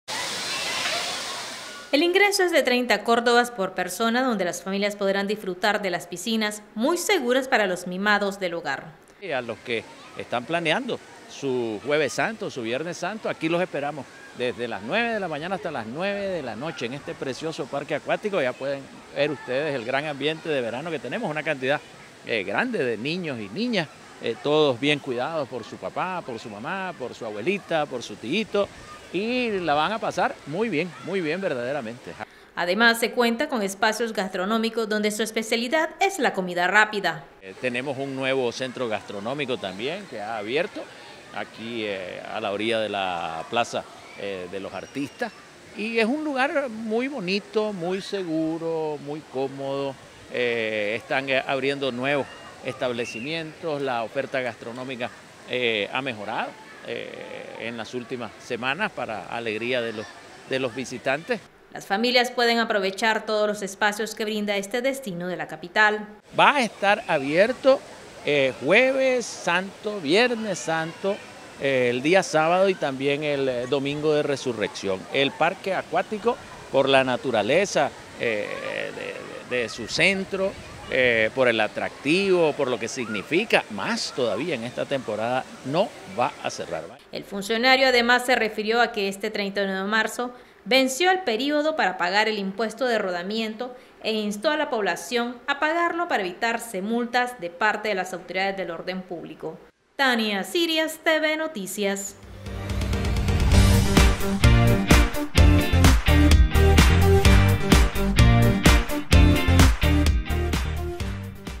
Vacacionar sin salir de Managua es posible en el parque acuático
Si quiere vacacionar sin salir de la ciudad, el parque acuático del paseo Xolotlán es una opción que usted debe tener en cuenta para estas fechas.